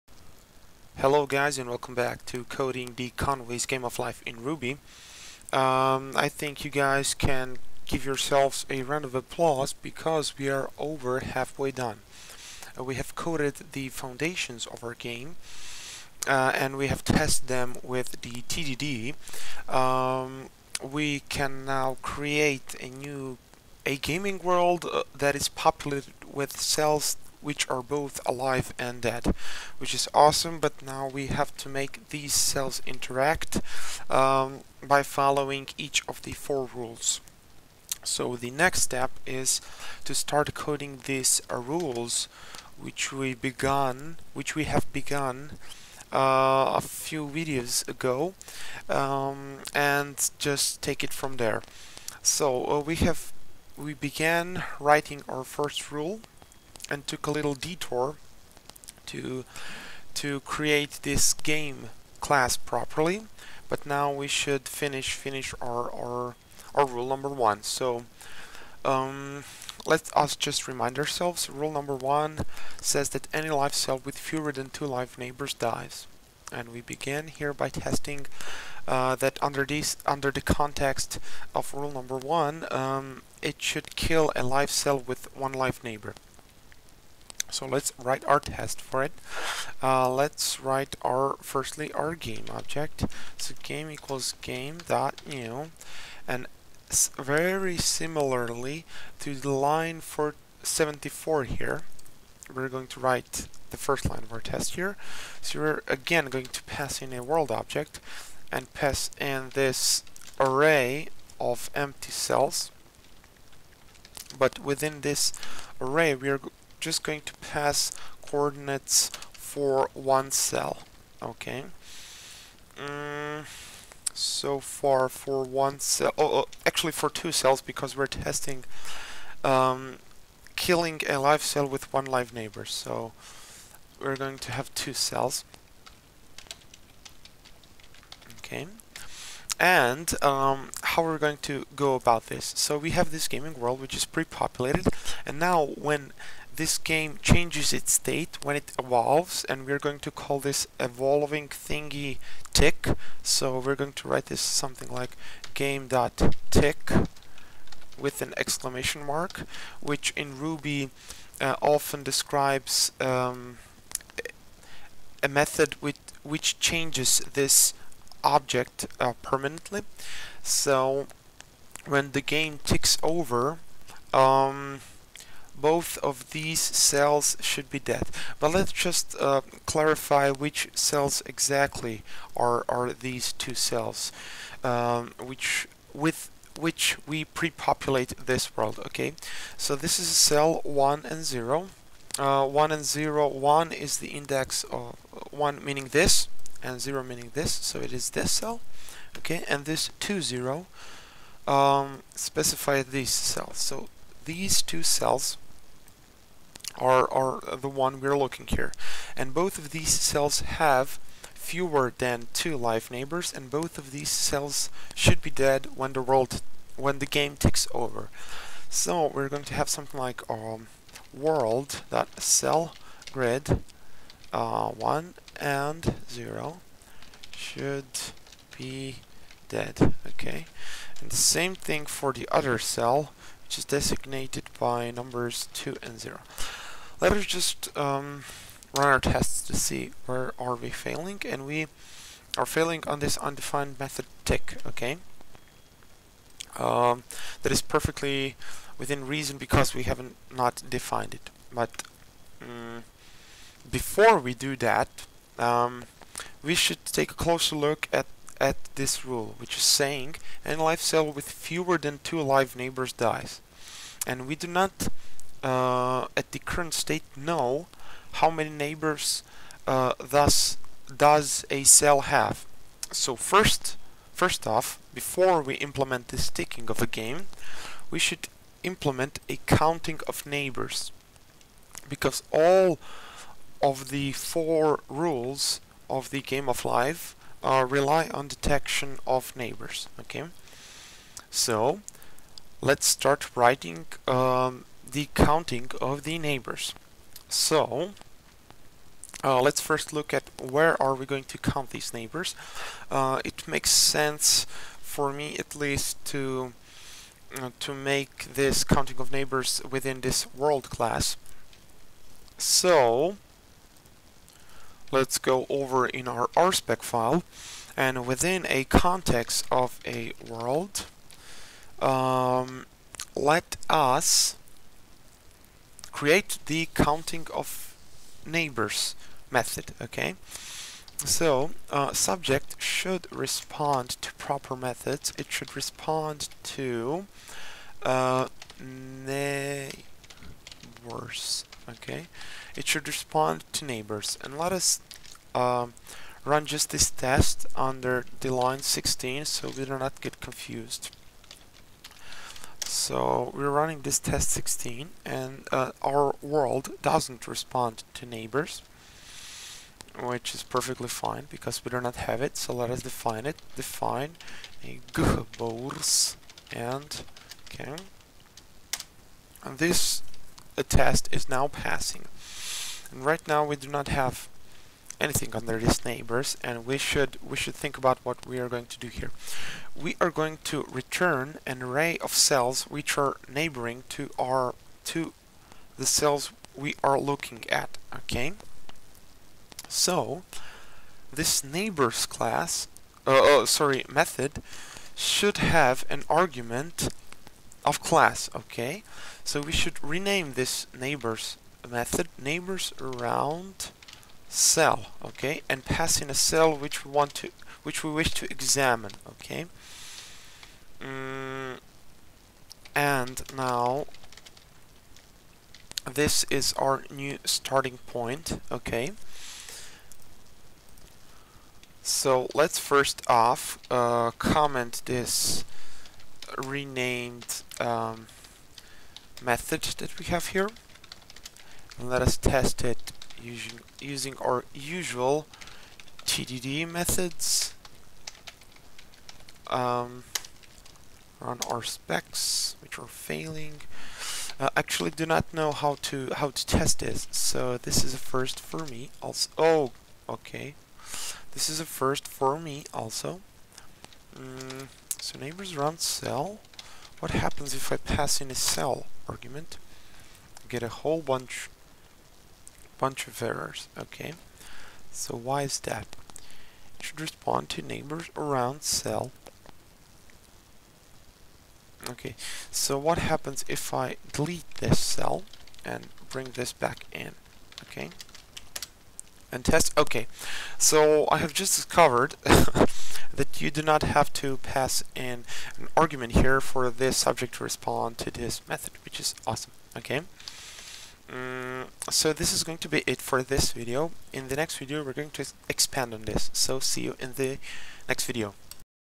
Game of Life in Ruby Tutorial - part 9 - Counting the neighbours (beginning)
Tutorial of coding the Conway's game of life in ruby programming language using TDD and Gosu gaming library.

Finished game is on GitHub, so feel free to check it out, fork it etc.